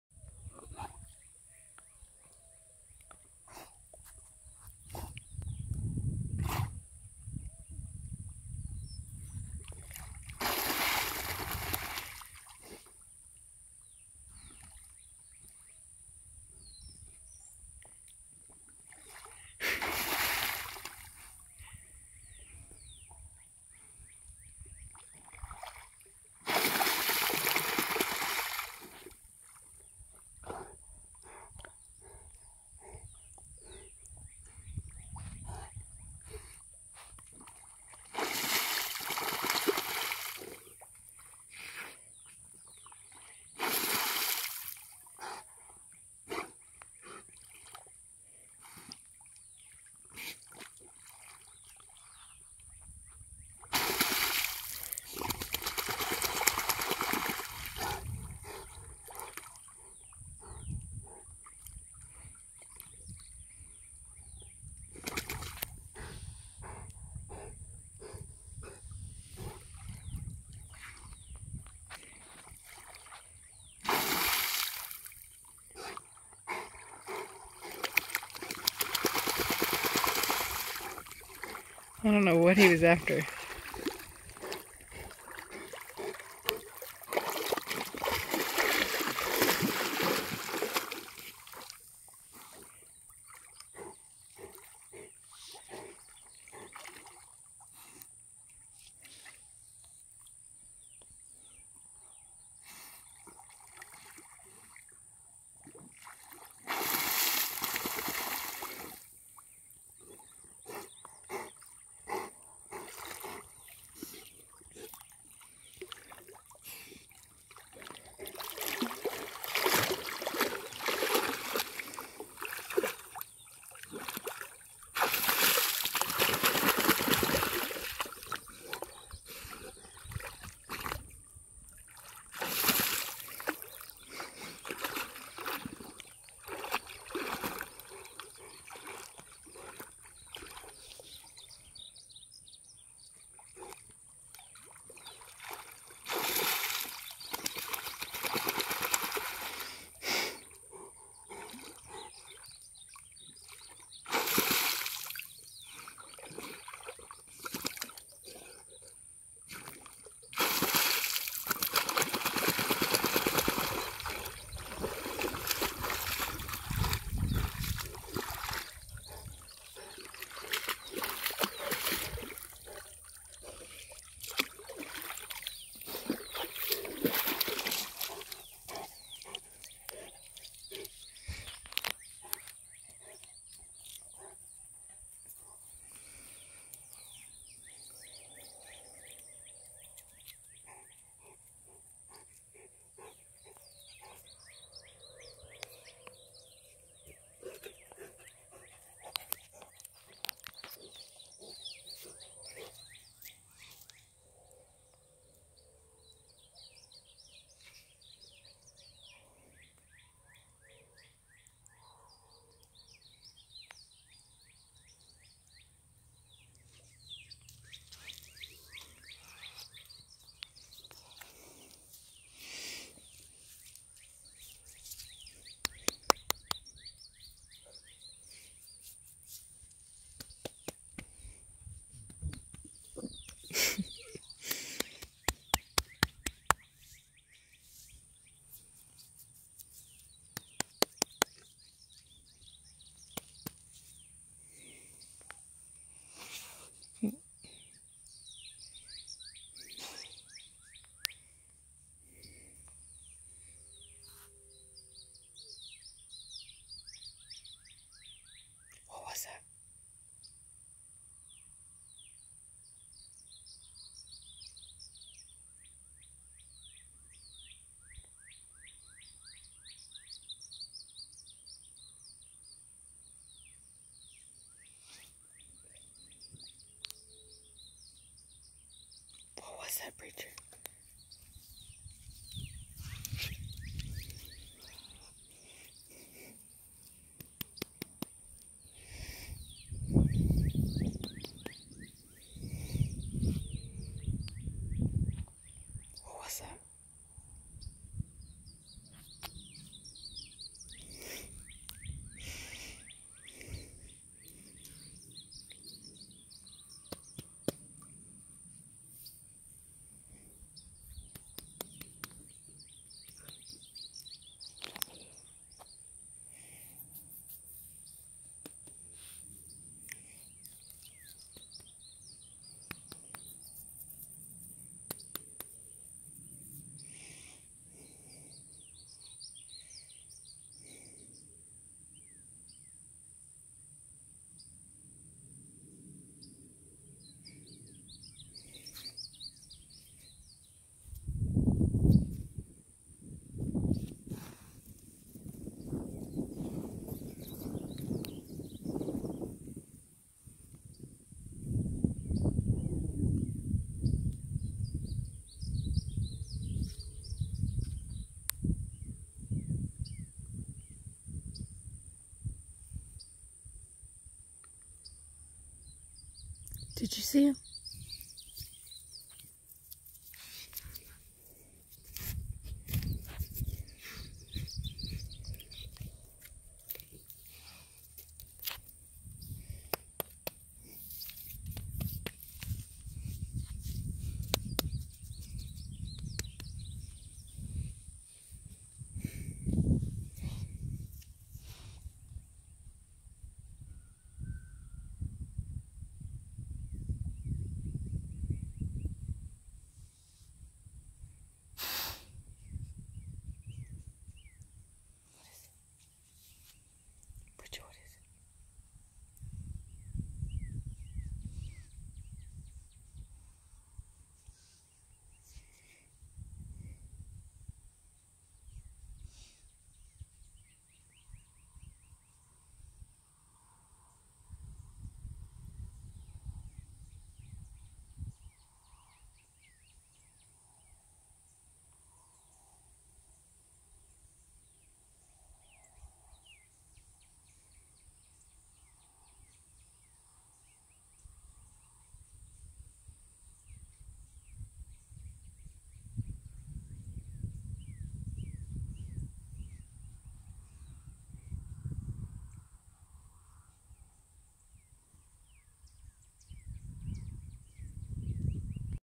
Diving Cane Corso
I think this is the farthest I've seen him go down yet. Lol

01198708abc987